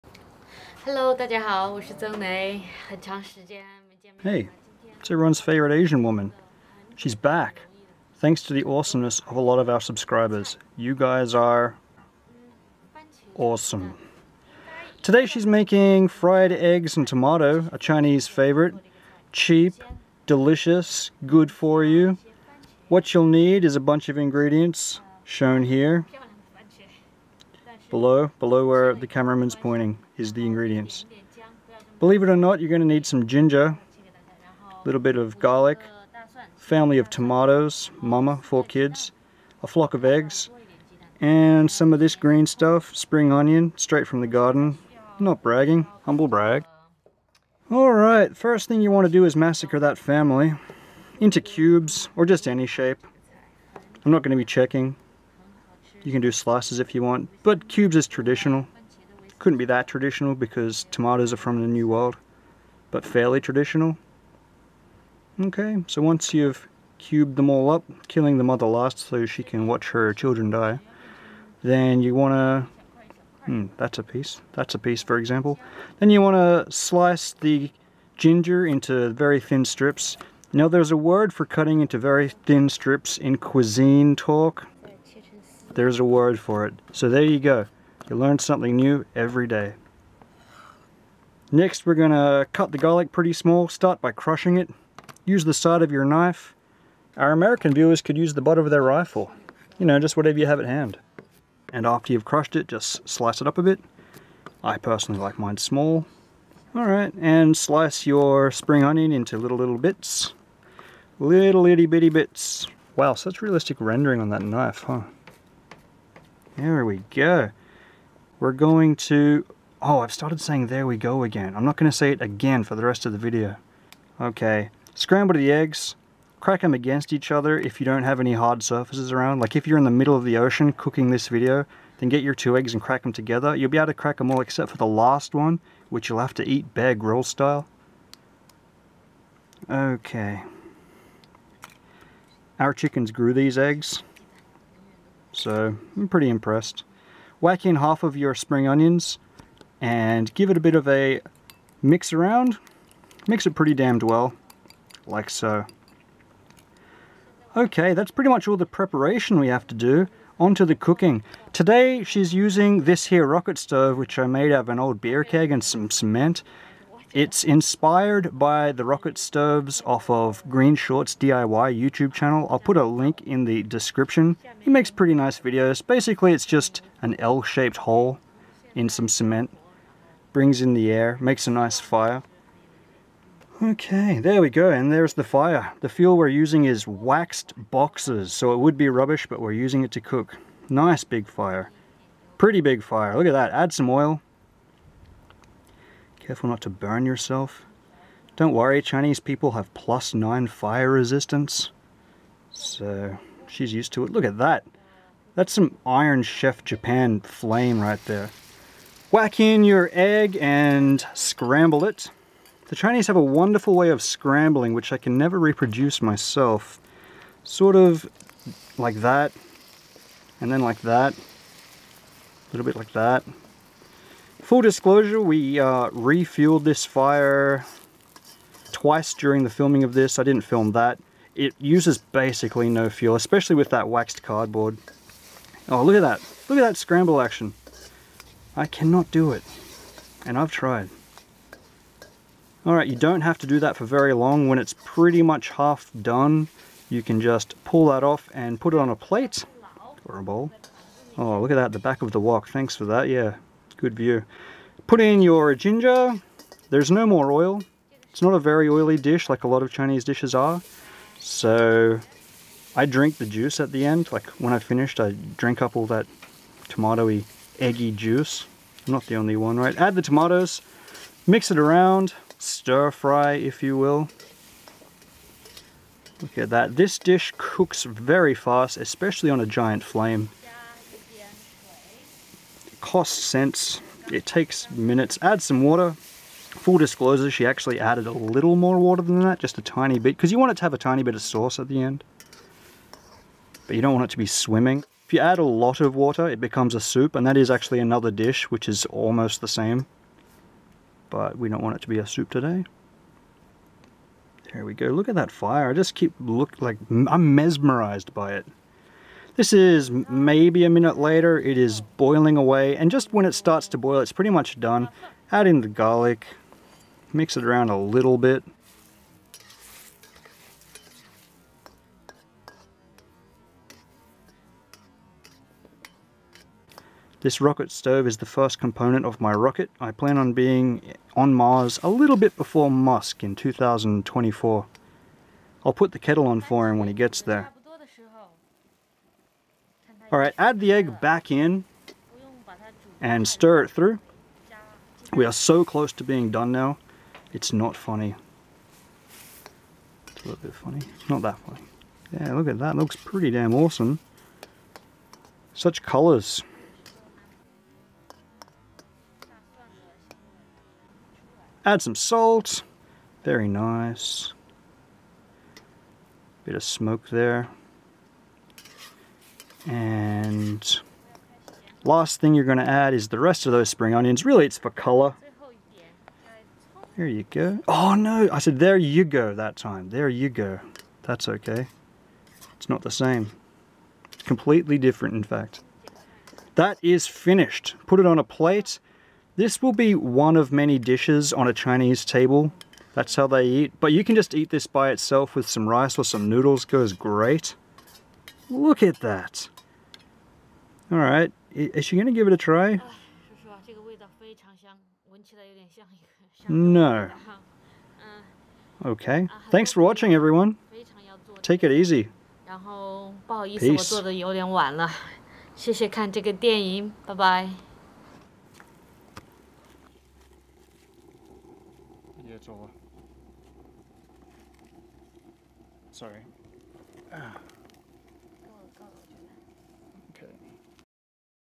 Tomato and egg
Chinese fried tomato and egg.
Ingredients: tomato, egg, garlic, ginger, spring onion, salt, oil.

Slap it together just like the video and you cant go wrong.
For a free pair of tennis shoes tell him sarcasmo57 sent you*. *Subject to availability, also I didn't tell him about this promotion.